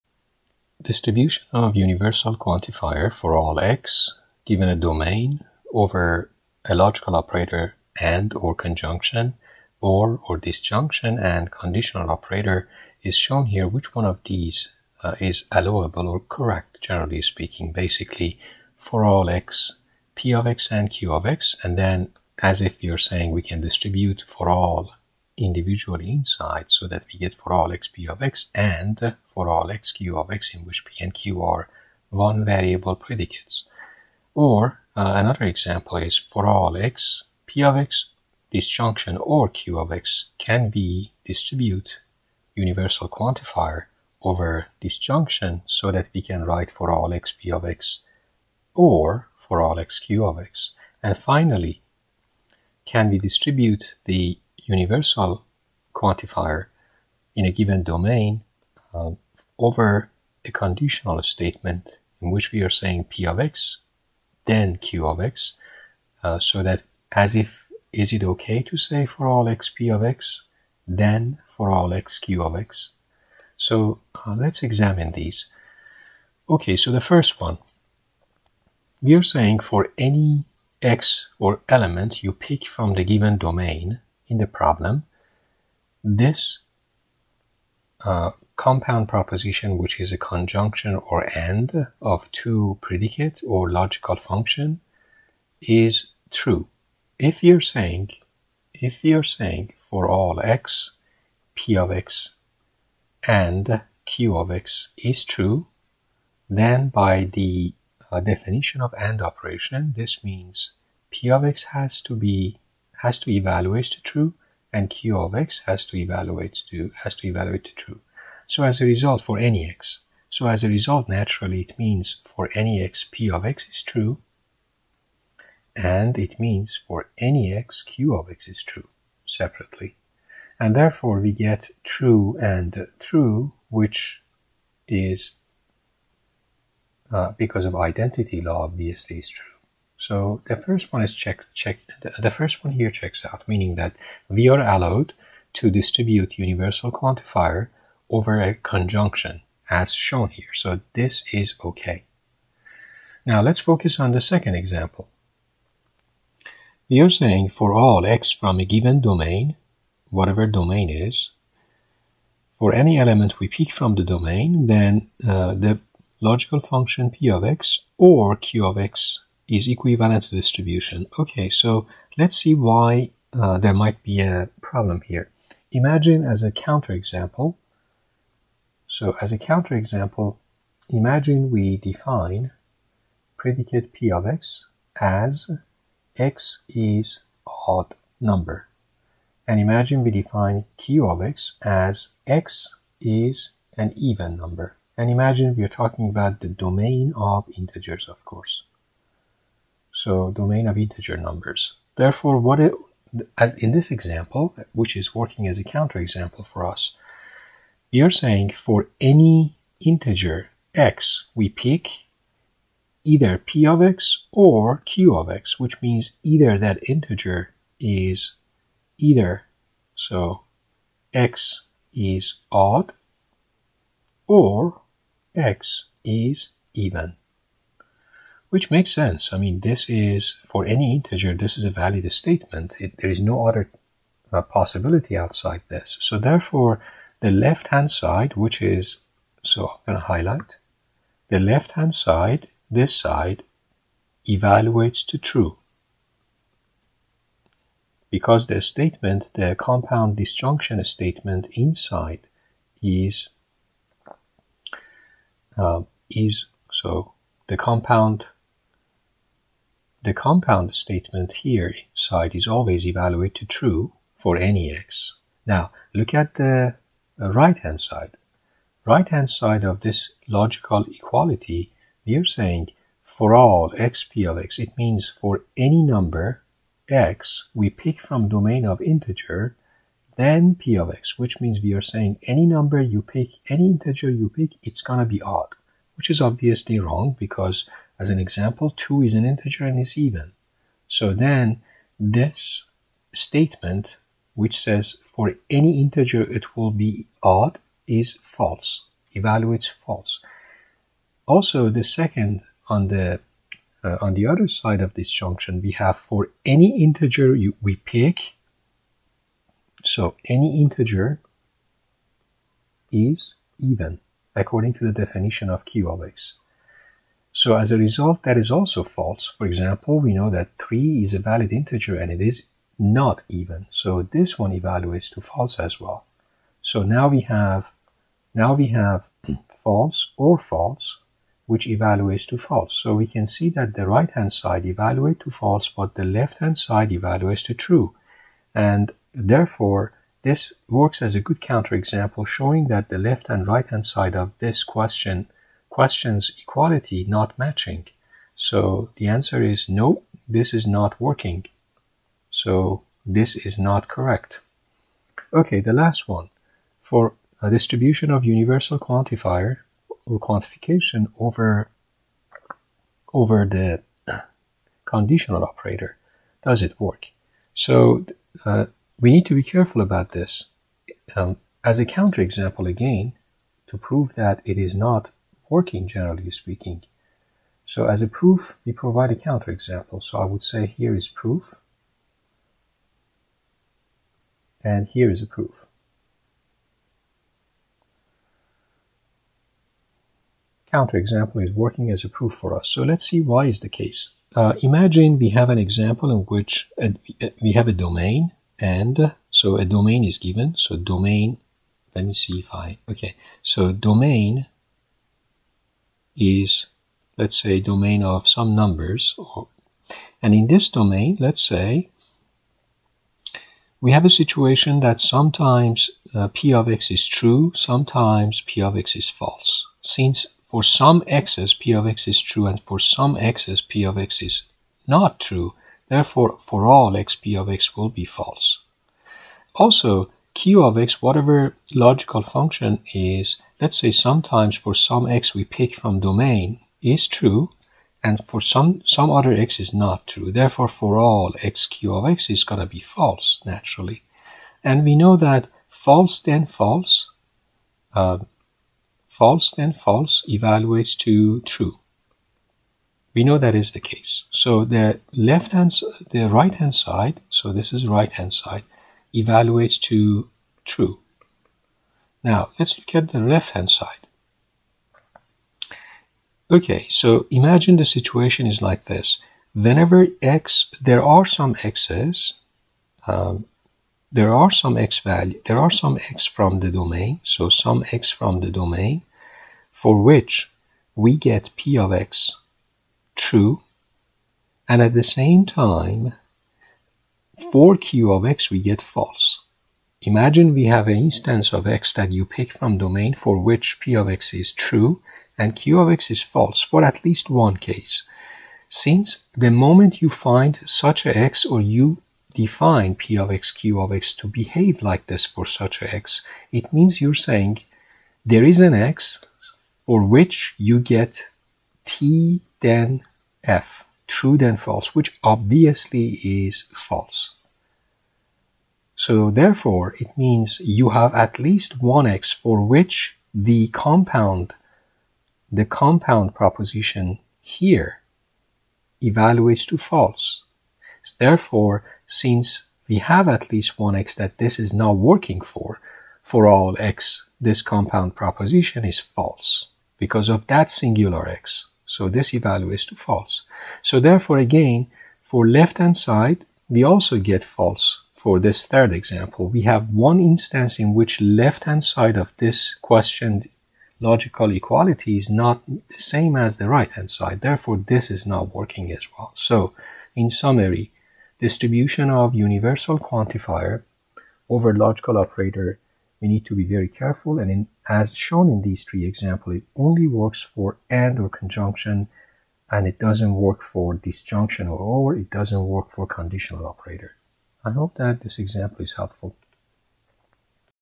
Logical Equivalence: Universal Quantifier Distribution
Does Universal quantifier distribute over AND operator (Conjunction) ?
Does Universal quantifier distribute over OR operator (Disjunction) ?
Does Universal quantifier distribute over Conditional operator (ternary operator) ?
These logical equivalence proof problems are examined in this video.  Counterexamples are provided wherever the universal quantification can't be distributed.
Stated alternatively:
   Is Universal quantification of conjunction equal to the conjunction of the universal quantifications ?
   Is Universal quantification of disjunction equal to the disjunction of the universal quantifications ?
   Is Universal quantification of conditional statement equal to the conditional universal quantifications ?